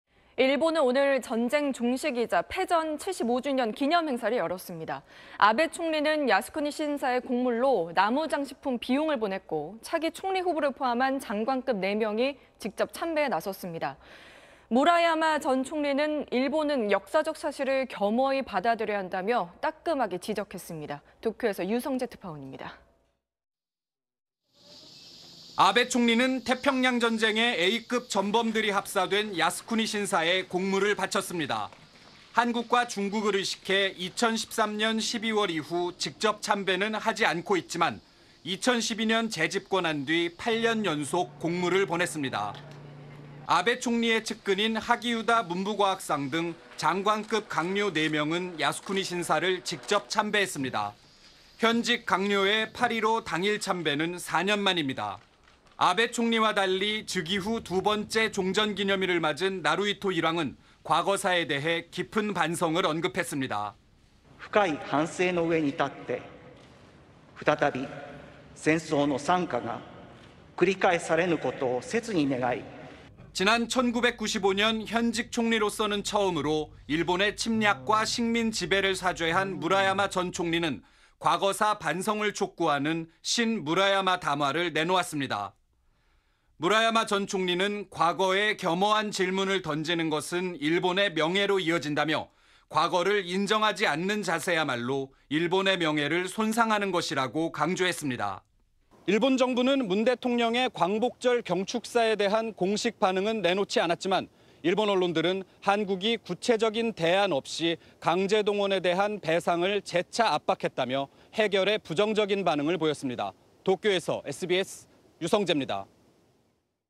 아베, 8년째 야스쿠니 공물…무라야마는 따끔한 일침 / SBS
일본은 오늘(15일) 전쟁 종식이자 패전 75주년 기념행사를 열었습니다. 아베 총리는 야스쿠니 신사에 공물로 나무장식품 비용을 보냈고, 차기 총리 후보를 포함한 장관급 4명이 직접 참배에 나섰습니다. 무라야마 전 총리는 일본은 역사적 사실을 겸허히 받아들여야 한다며 따끔하게 지적했습니다.
도쿄 유성재 특파원입니다.